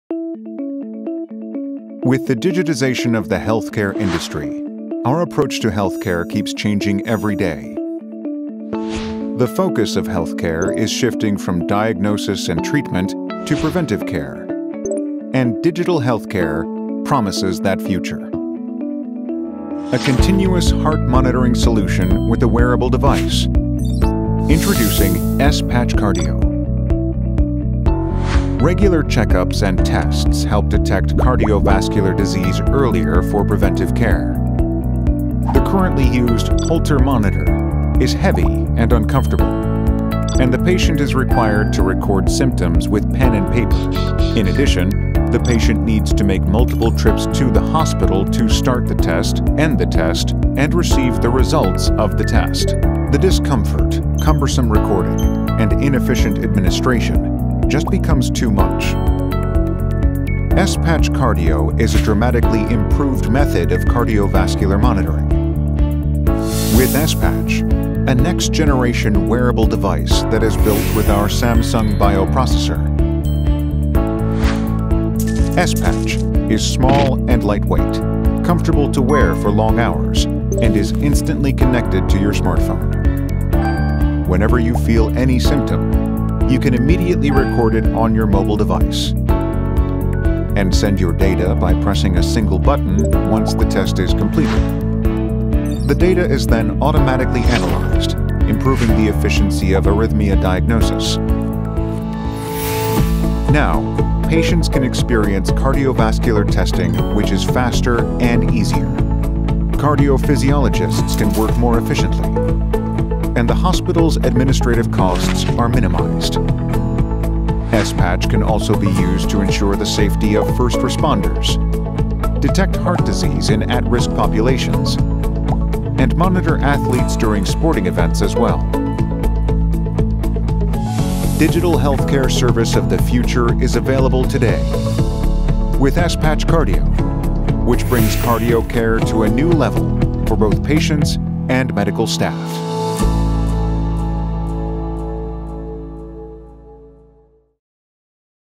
Wearable ECG(Electrocardiogram) Monitoring Solution, S-Patch Cardio
Monitoring cardiovascular diseases can be made easier through S-Patch Cardio of Wellysis, a spinoff company of Samsung SDS. With a simple wearable device called the "S-Patch", halter monitors and manually transcribing symptoms are made easier.